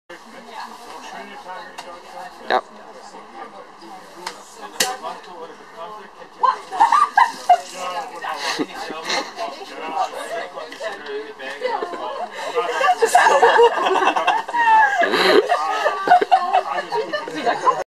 Start in den Tag mit Sekt in der Deutschen Bahn (DB)
Auf dem Weg nach Hamburg lassen wir es uns gut gehen